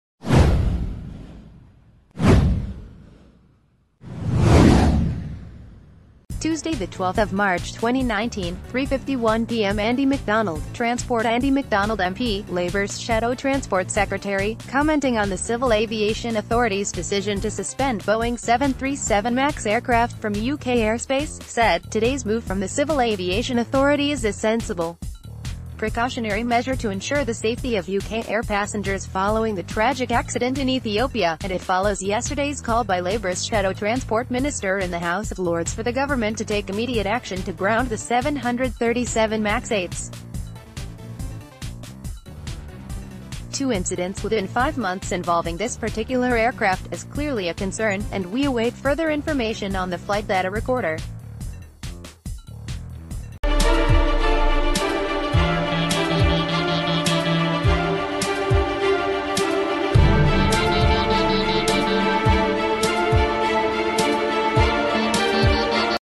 Andy McDonald responds to CAA announcement to suspend Boeing 737 Max aircraft UK airspace - The L...
Andy McDonald responds to CAA announcement to suspend Boeing 737 Max aircraft UK airspace - The Labour Party

Tuesday 12 March 2019 / 3:51 PM Andy McDonald / Transport Andy McDonald MP, Labour’s Shadow Transport Secretary, commenting on the Civil Aviation Authority’s decision to suspend Boeing 737 Max aircraft from UK airspace, said: “Today’s move from the Civil Aviation Authority is a sensible precautionary measure to ensure the safety of UK air passengers following the tragic accident in Ethiopia, and it follows yesterday’s call by Labour’s Shadow Transport Minister in the House of Lords for the gover...